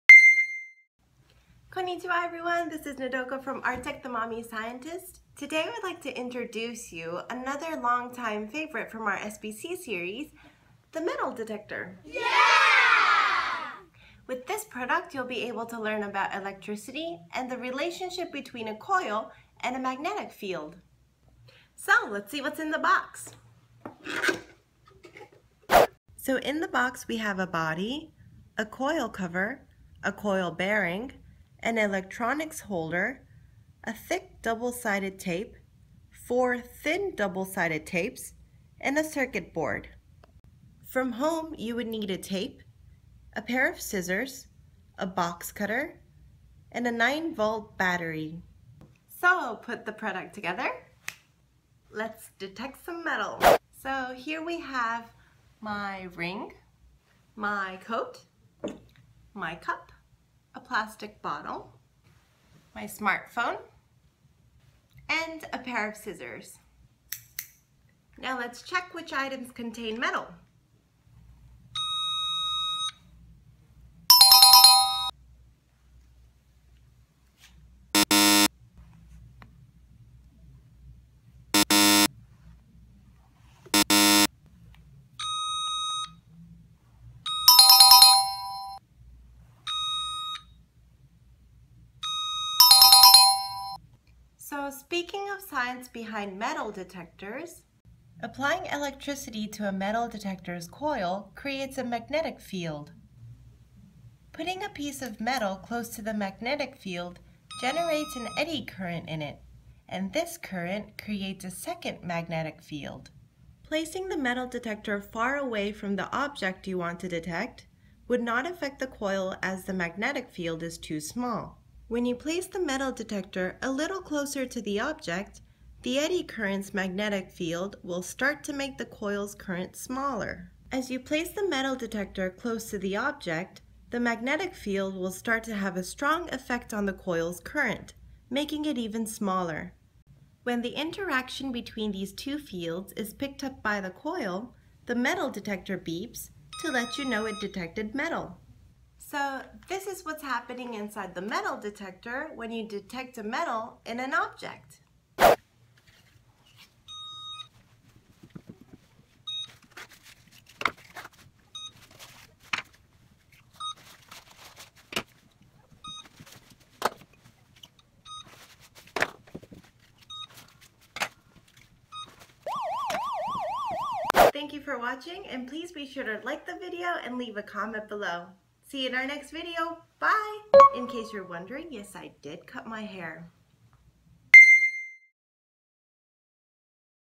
SPC MetalDetector -product video- by Artec Co., Ltd.
Another long-time-favorite from our SPC series, the Metal Detector!!
With this product, you would be able to learn about electricity and the relationship between a coil and a magnetic field!

Watch me experiment on different items and also do a security check!

Artec Co., Ltd. is the leading educational materials manufacturer in Japan.